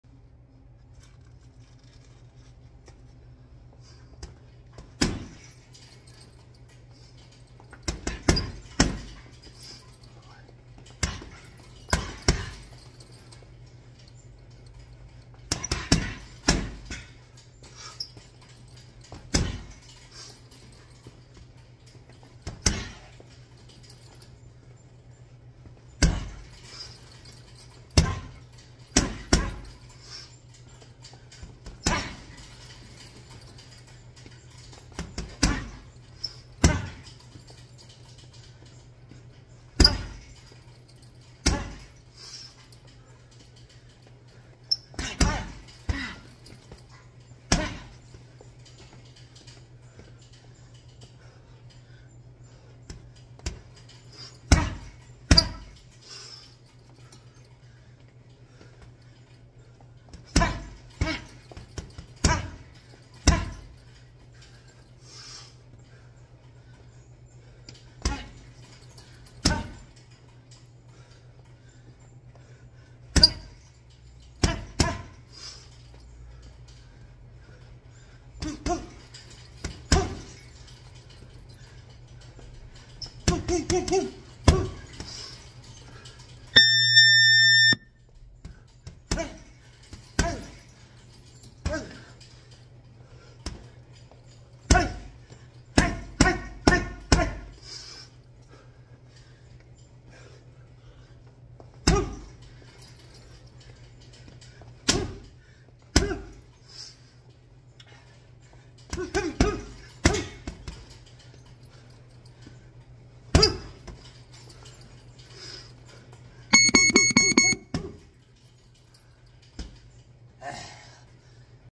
Back in the lab, 185Lbs
gained a bit of weight since my surgery but iam back at it 185 weight class!